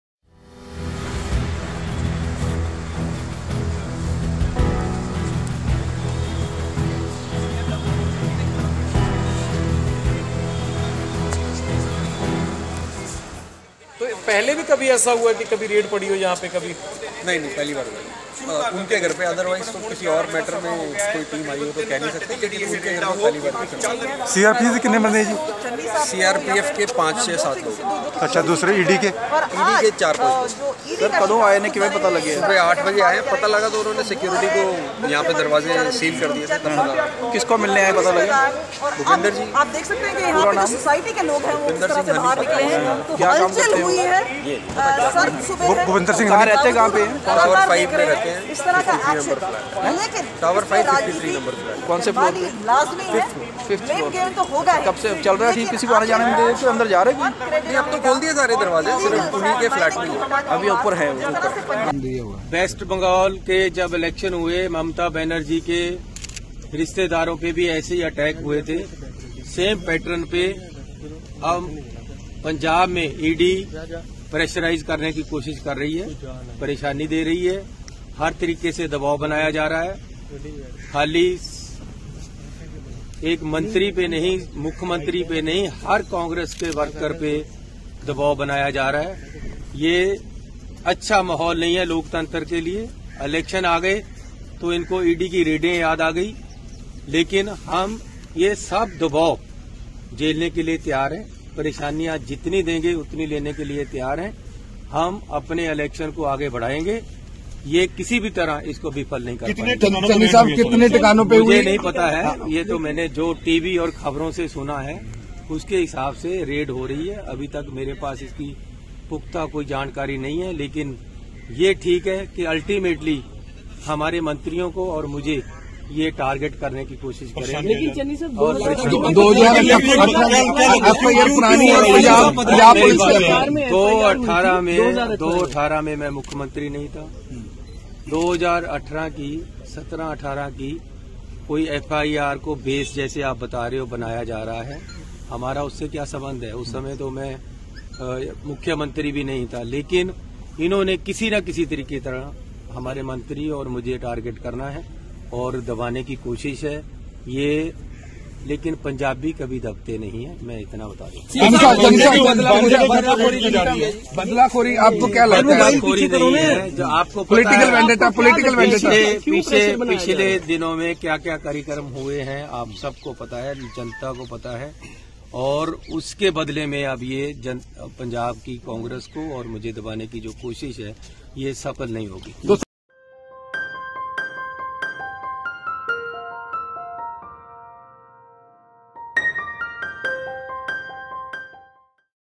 Illegal sand mining: ED raids multiple locations in Punjab, including that of CM Channi's relative
The Enforcement Directorate on Tuesday raided the flat of Bhupinder Singh Honey, said to be Punjab Chief Minister Charanjit Singh Channi’s nephew, at Homeland Heights here in an illegal land mining case.

According to sources, the raids were being conducted in 19 locations in Punjab by the enforcement directorate and the action is being taken under the provisions of the Prevention of Money Laundering Act (PMLA).

However, there was no official response so far.